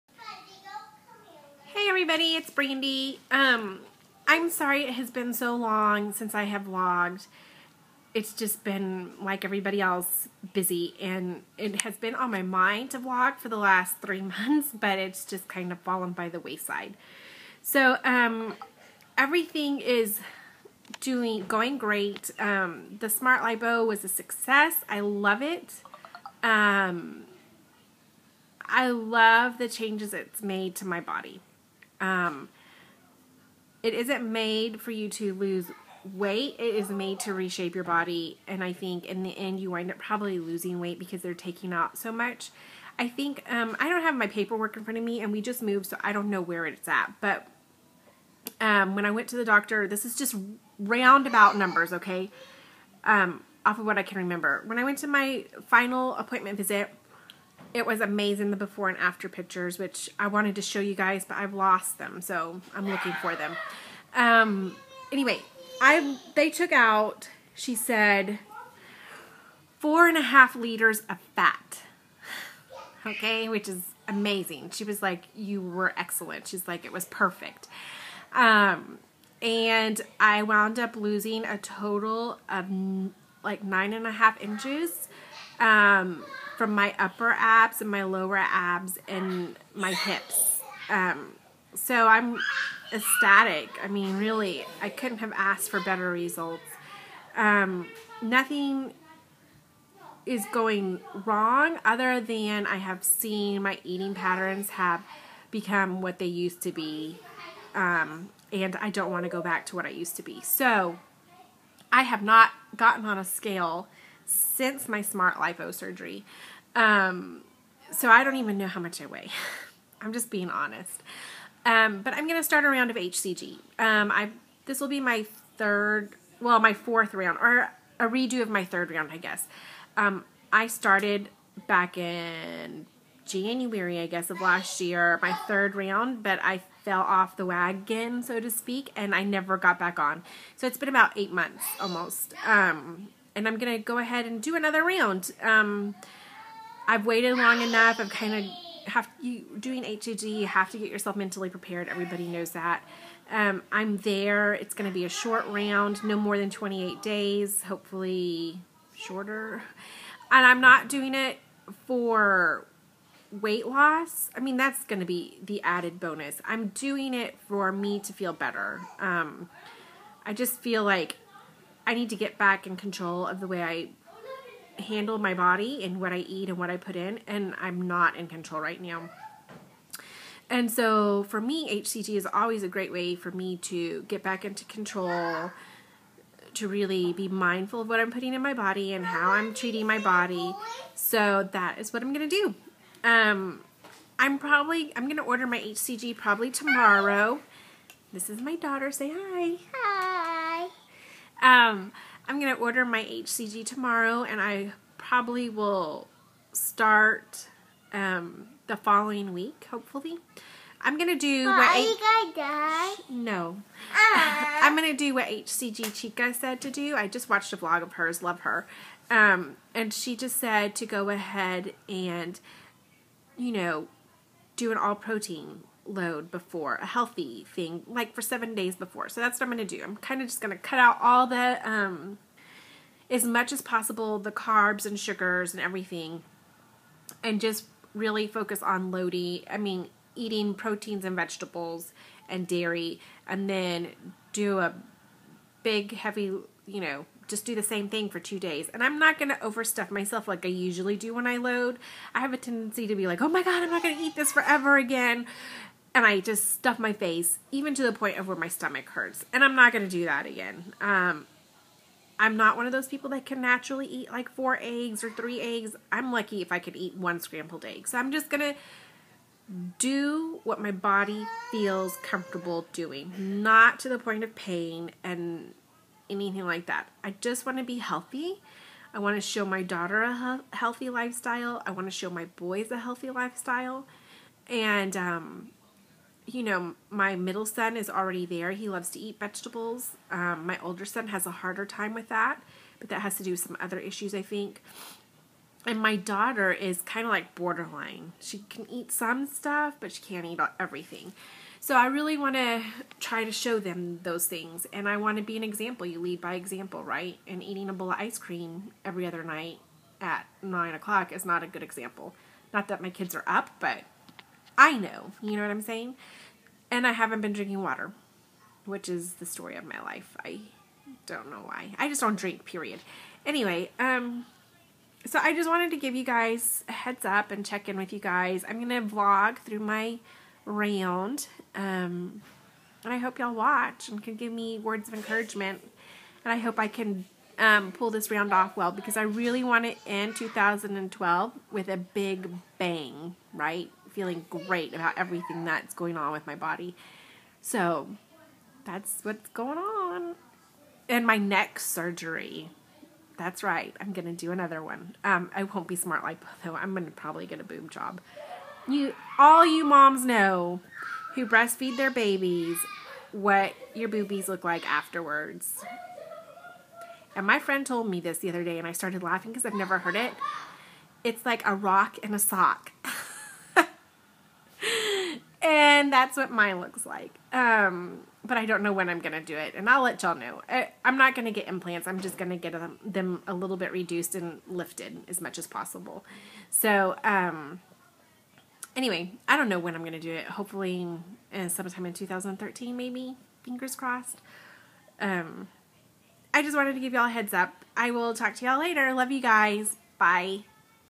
Check in
Update on my smart lipo and getting ready for a round hcg